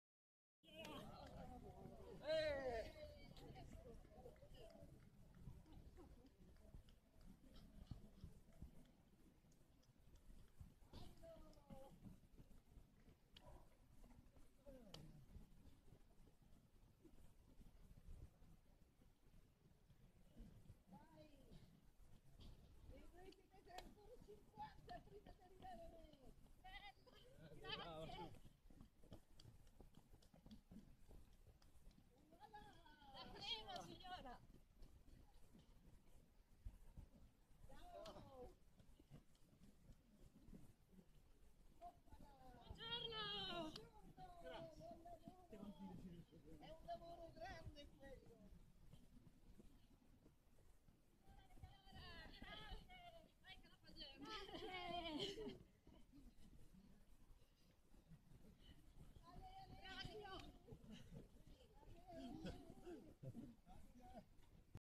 Une mamie fait des high five pendant un marathon
Dimanche dernier s'est déroulée la vingtième édition du marathon de Ravenne, en Italie. Sur le parcours, une vieille dame s'est tenue debout au bord de la route et a fait des high five d'encouragement à plusieurs coureurs qui passaient à son niveau. Sympa !